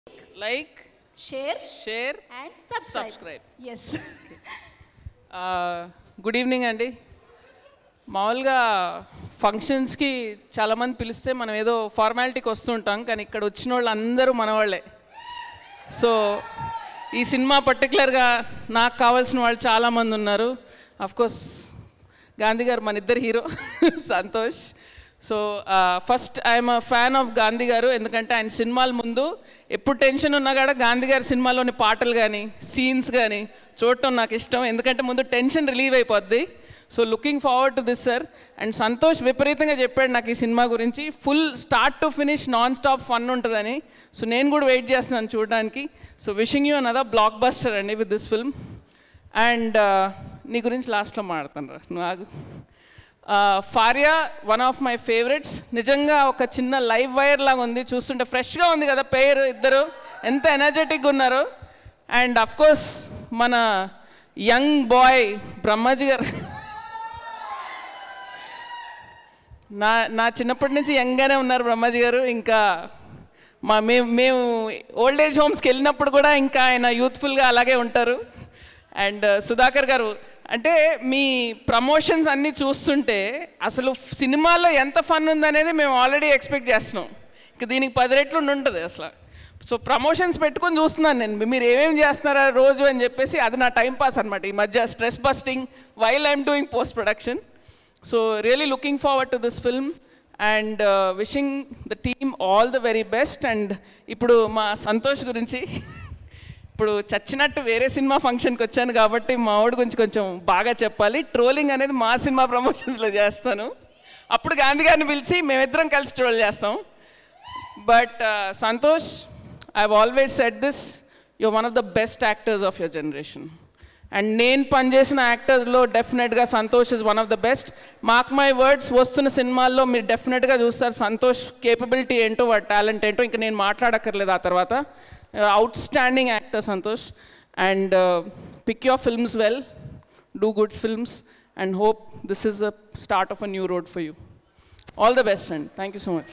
Brahmaji ని ఓ ఆట ఆదుకున్న లేడీ డైరెక్టర్ *Tollywood | Telugu FilmiBeat
The movie is directed by Merlapaka Gandhi and will feature Santosh Shoban, Faria Abdullah, Brahmaji and Mime Gopi as lead characters. ఈ చిత్రానికి మేర్లపాక గాంధీ దర్శకత్వం వహించారు మరియు సంతోష్ శోభన్, ఫరియా అబ్దుల్లా, బ్రహ్మాజీ మరియు మైమ్ గోపి ప్రధాన పాత్రల్లో నటించారు. లైక్, షేర్ & సబ్‌స్క్రైబ్ కోసం ఎంపికైన ఇతర ప్రముఖ నటులు గోవింద్ పద్మసూర్య మరియు సప్తగిరి.